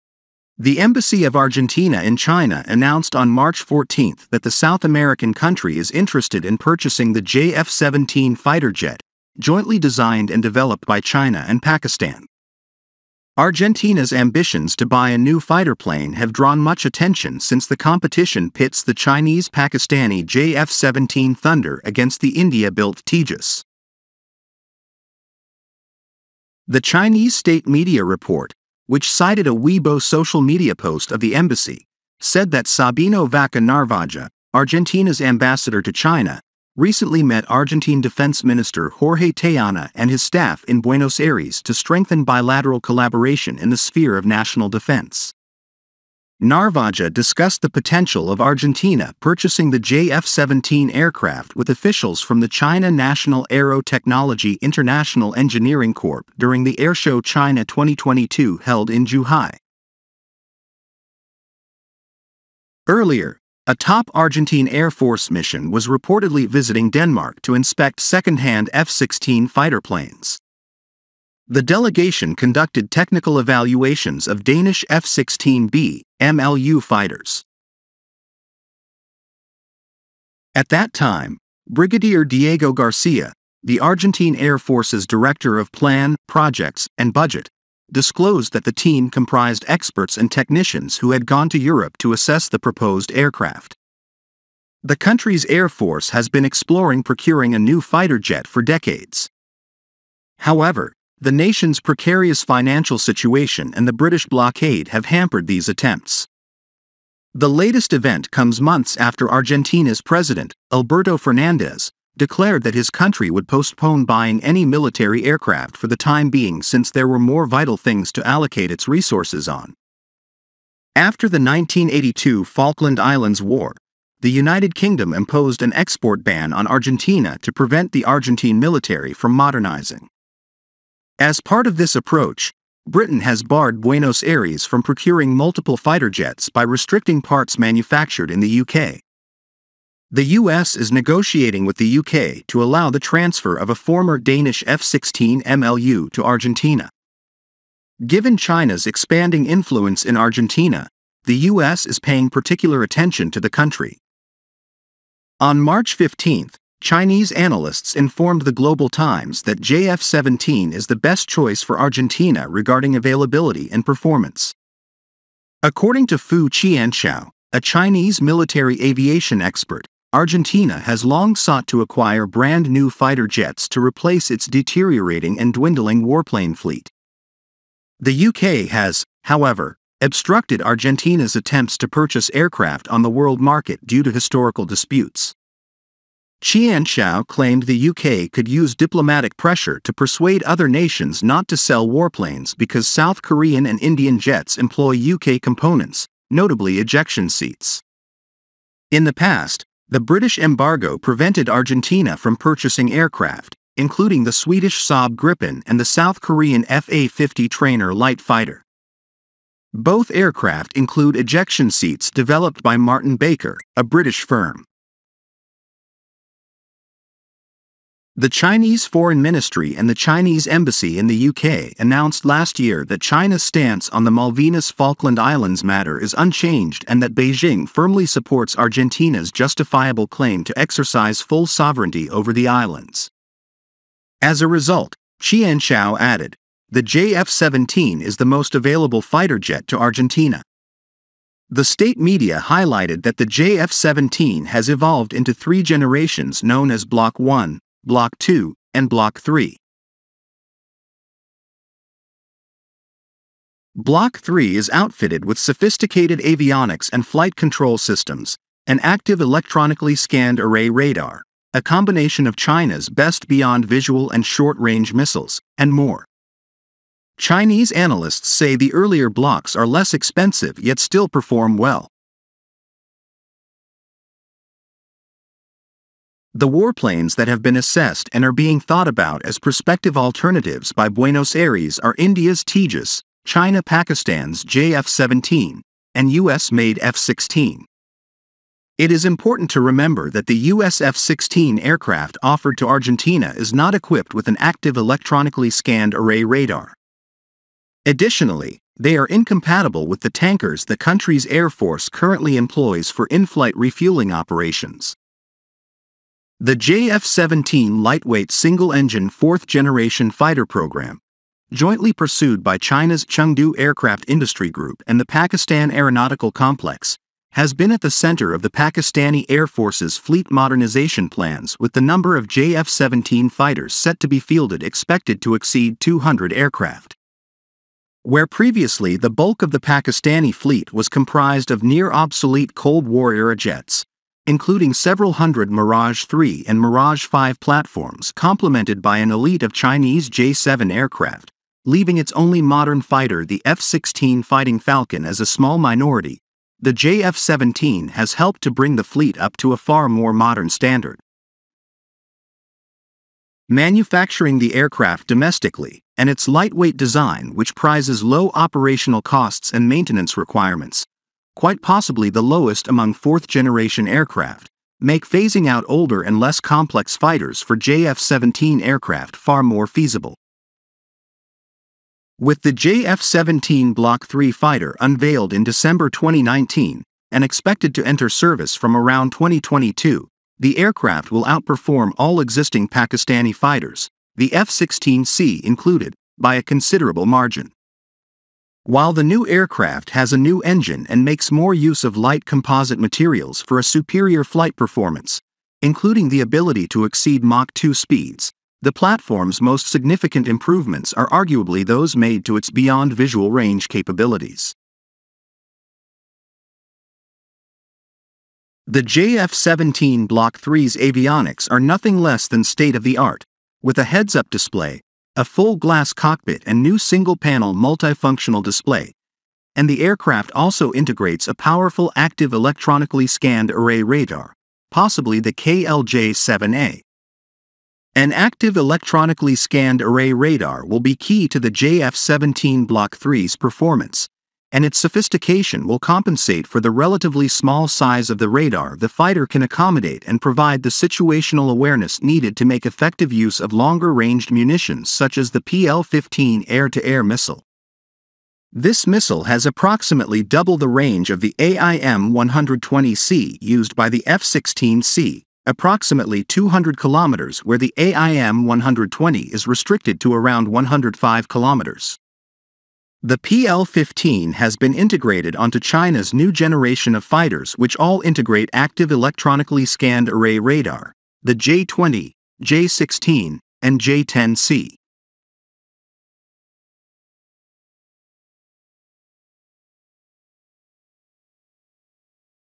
China’s Super JF-17 To Seal Argentina Fighter Jet Deal, US F-16, Indian LCA Tejas Unsuitable
China’s Super JF-17 To Seal Argentina Fighter Jet Deal, US F-16, Indian LCA Tejas Unsuitable, The JF-17 is the most available fighter jet to Argentina. The State media highlighted that the JF-17 has evolved into three generations known as Block 1, Block 2, and Block 3.